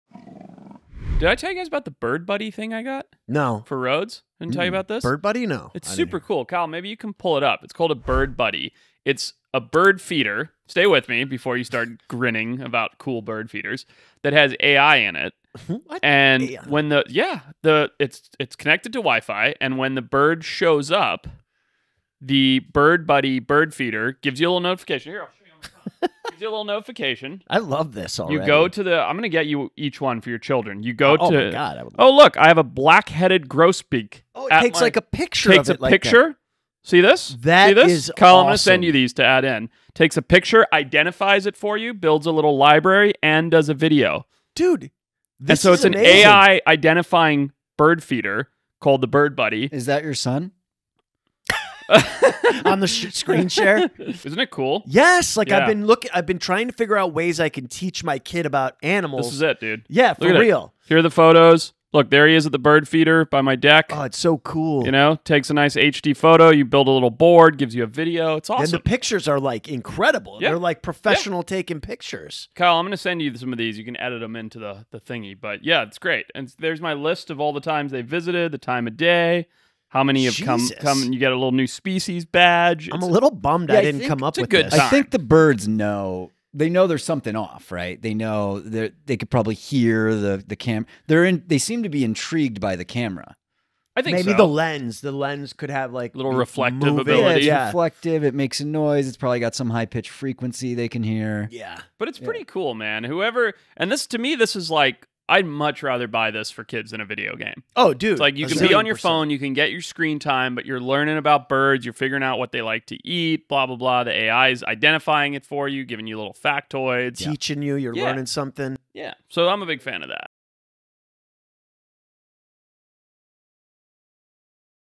AI Bird Feeder Can Identify Your Birds?!
The Bird Buddy bird feeder has built-in AI that can keep track of birds that visit you!